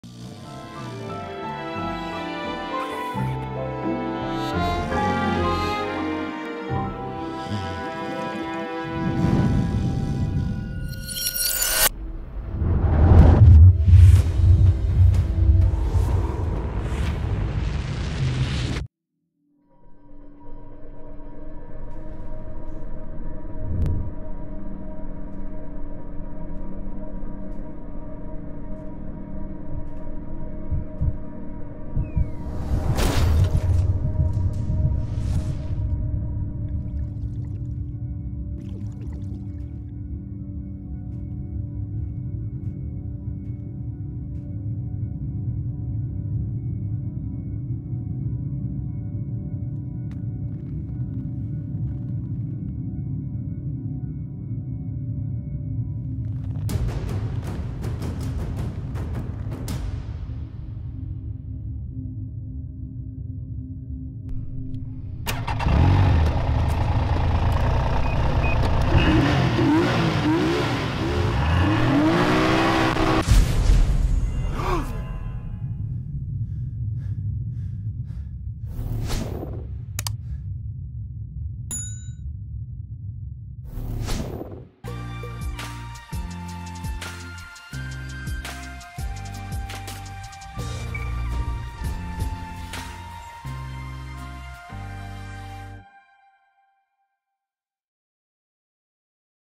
Men´s Dream / Männer (Alb)Traum -Lynx Alpine Skidoo - A short Sony A7 III with Tamron 28-75 Video
A Short clip Shot on my new Sony A7 III with Tamron 28-75
Riding a Lynx Skidoo

sony a7iii
tamron 28-75